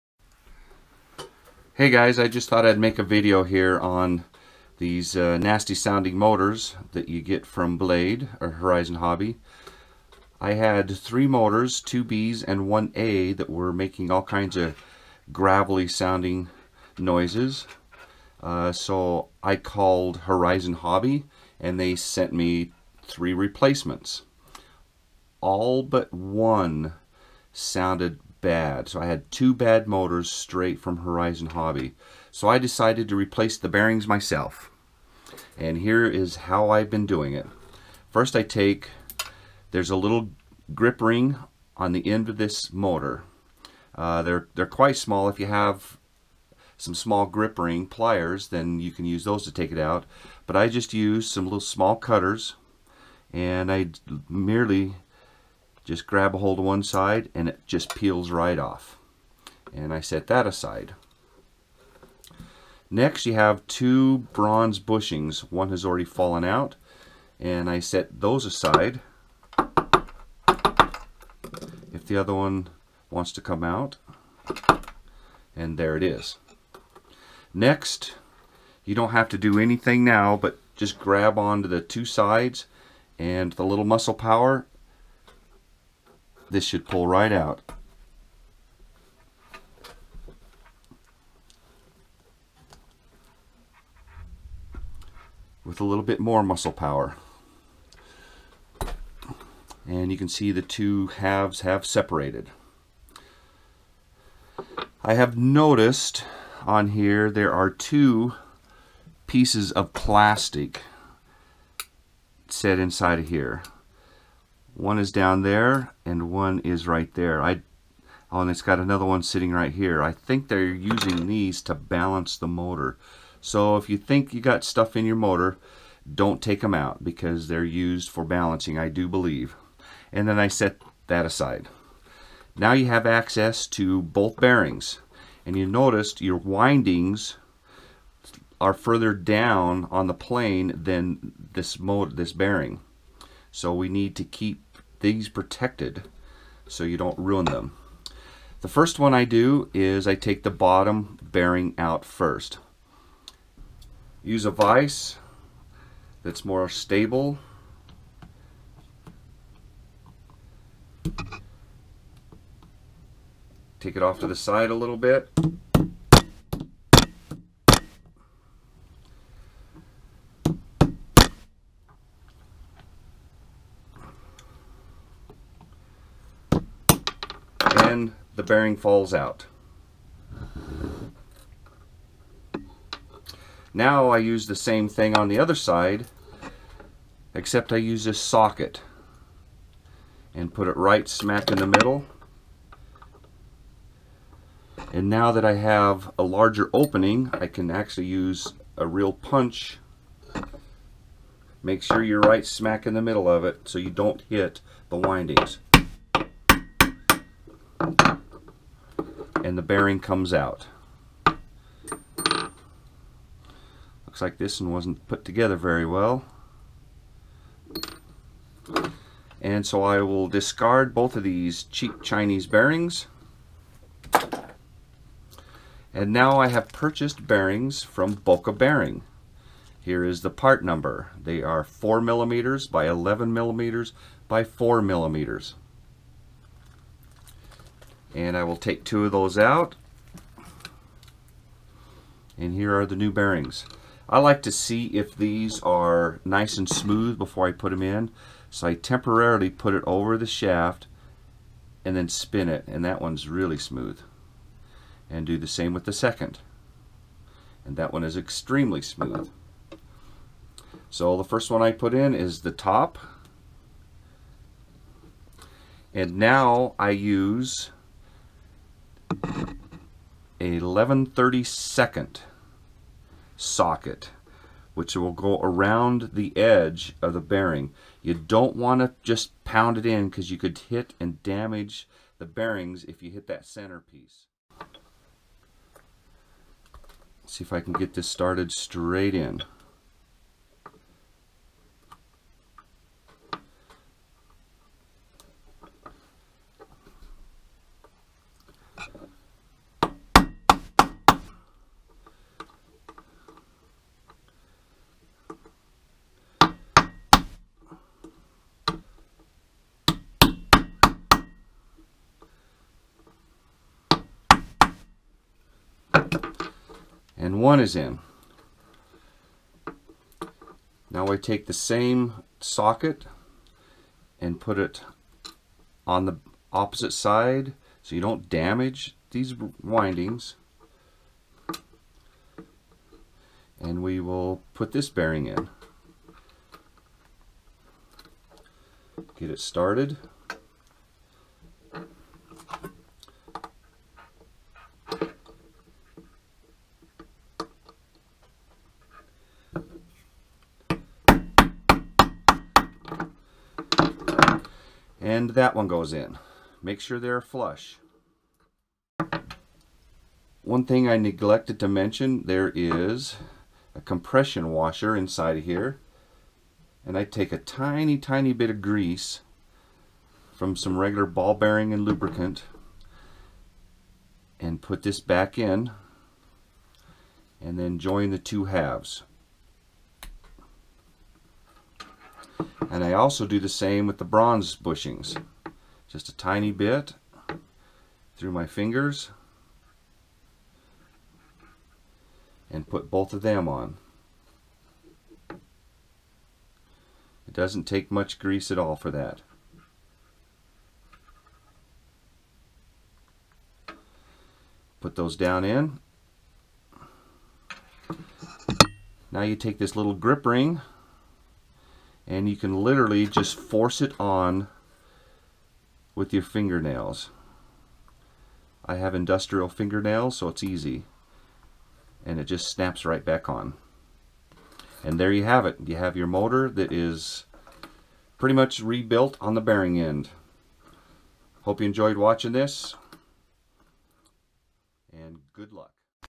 Chroma Quad bearing replacement
Short video on how to replace the bearings on your new motor that are making noise.